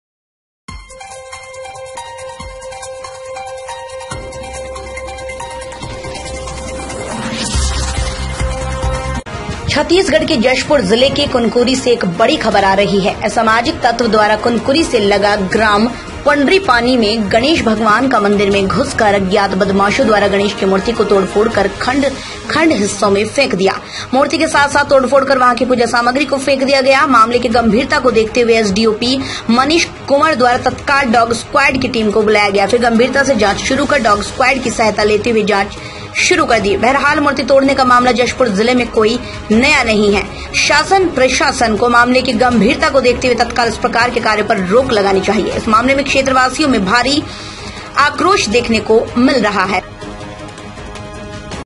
कुनकुरी/जशपुर :असमाजिक तत्व ने तोड़ा गणेश की मूर्ति, लोगों में आक्रोश, पुलिस जांच में जुटी
असमाजिक तत्व ने तोड़ा गणेश की मूर्ति, लोगों में आक्रोश, पुलिस जांच में जुटी